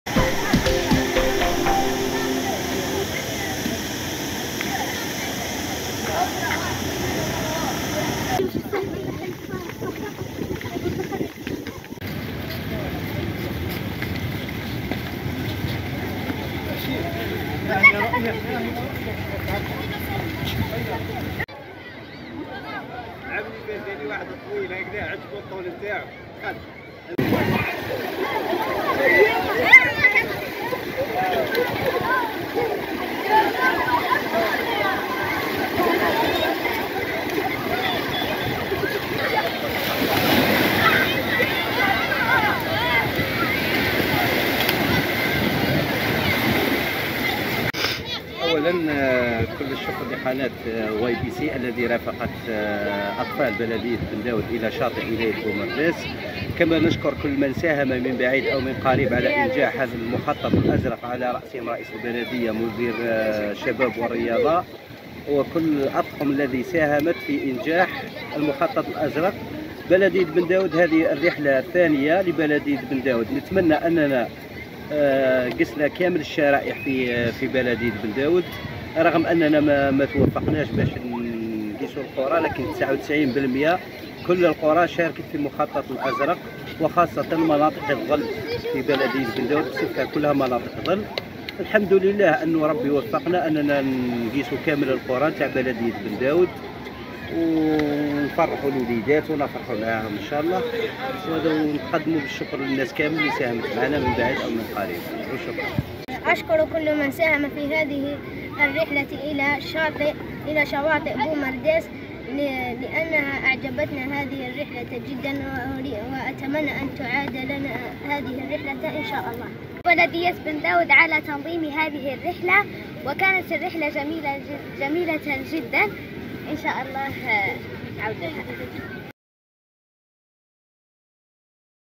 برج بوعريريج: مشاركة أبناء بلدية بن داود لبرنامج المخطط الأزرق الصيفي لسنة 2023
بومرداس / سياحة
مشاركة أبناء بلدية بن داود لبرنامج المخطط الأزرق الصيفي لسنة 2023